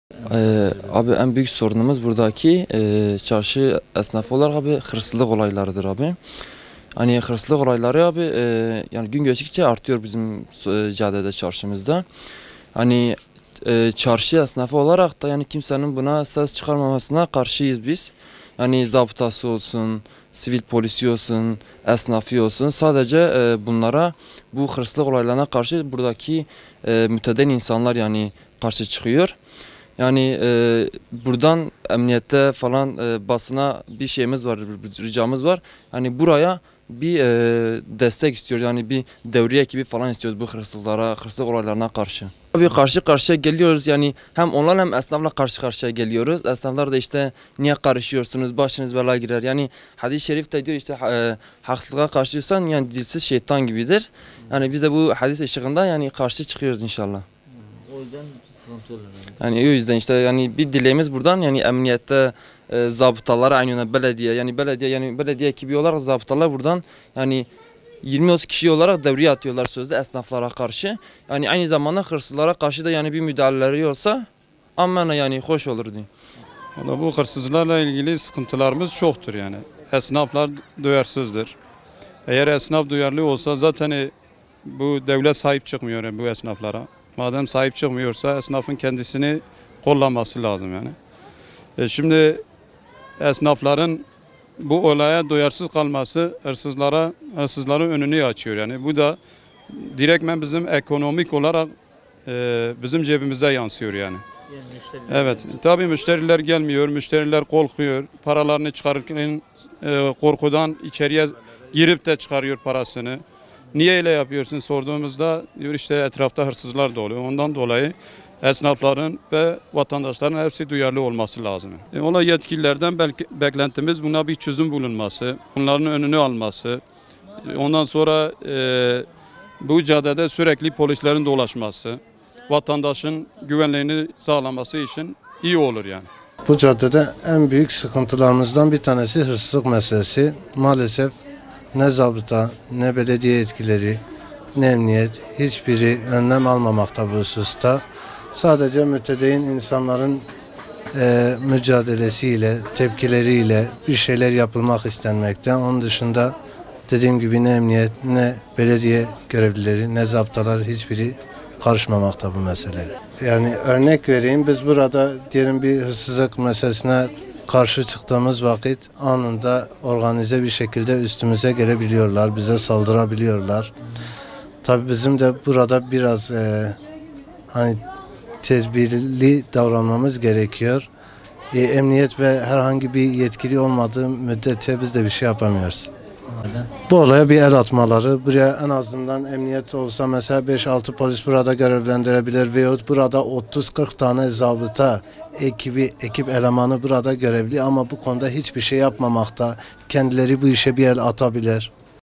Hırsızlara karşı biran önce tedbir alınsın \ 05 07 2014 \ DİYARBAKIR
Son zamanlarda Diyarbakır'da ardı arkası kesilmeyen hırsızlık olayları, Diyarbakır esnafını adeta çileden çıkardı. Esnaf, biran önce hırsızlığın önüne geçilebilmesi için gereken tedbirlerin alınmasını istiyor.